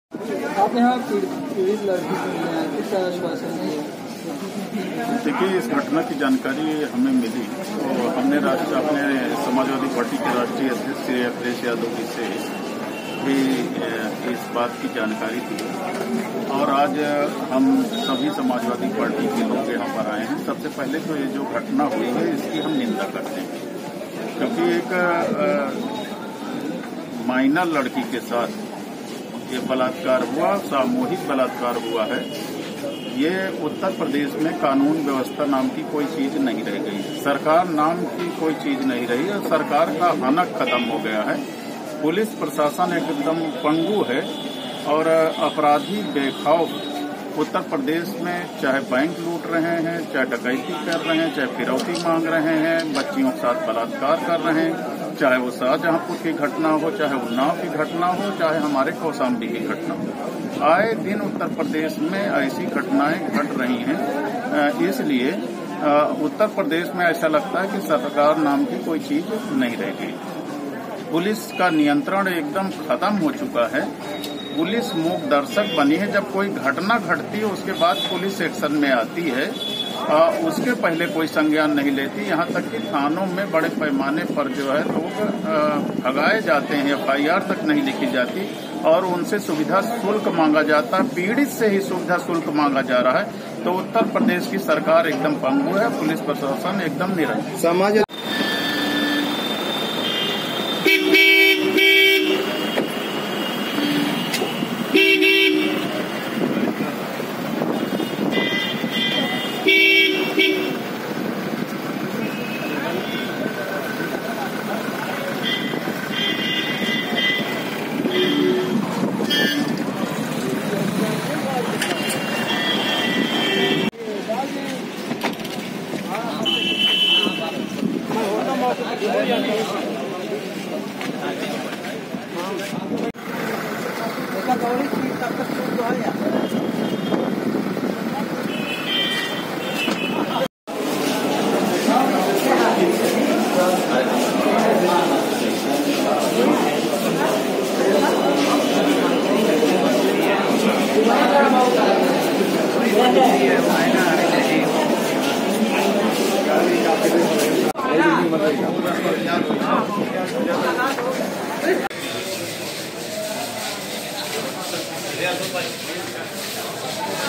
Kaushambi Gangrape Case: SP नेता Indrajeet Saroj ने की पीड़िता से मुलाकात
कौशांबी जिले में 21 सितंबर को हुए सामूहिक दुष्कर्म मामले में बुधवार को सपा के राष्ट्रीय महासचिव इन्द्रजीत सरोज़ पीड़िता व उनके परिवार से मिलने जिला अस्पताल पहुंचे। परिवार से मिलकर उनका दुःख बांटा, साथ ही हर संभव मदद करने को कहा । उन्होंने मीडिया कर्मियों से बात करते हुए सामूहिक दुष्कर्म की घटना की कड़ी निंदा करते हुवे भाजपा सरकार को अड़े हाथों लिया। उन्होंने कहा कि उत्तर प्रदेश में कानून नाम की कोई चीज़ नहीं रह गयी है । सरकार की हनक खात्म हो गयी है । पुलिस प्रशासन एकदम पंगु हो चुका है।